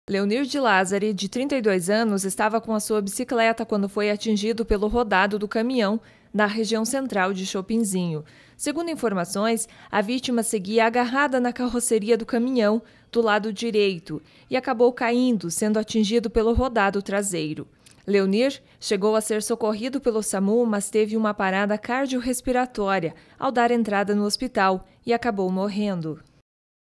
Jovem morre após ser atingido por rodado de caminhão
Fatalidade: Na tarde de domingo (06), um rapaz morreu após ter sido atingido pelo rodado de um caminhão, no município de Chopinzinho.